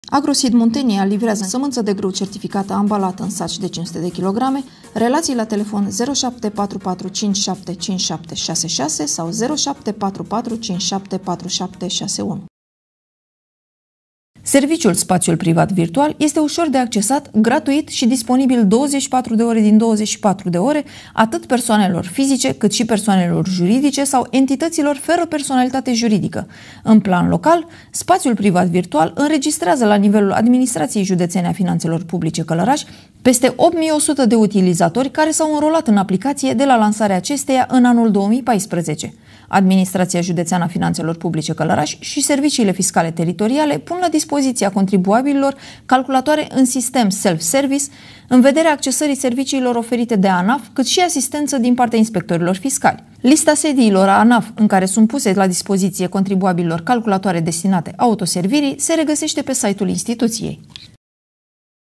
Știri Locale - Utilizatori înscriși în Spaţiul Privat Virtual
Serviciul Spaţiul Privat Virtual este uşor de accesat, gratuit şi disponibil 24 de ore din 24, atât persoanelor fizice, cât şi persoanelor juridice sau entităţilor fără personalitate juridică.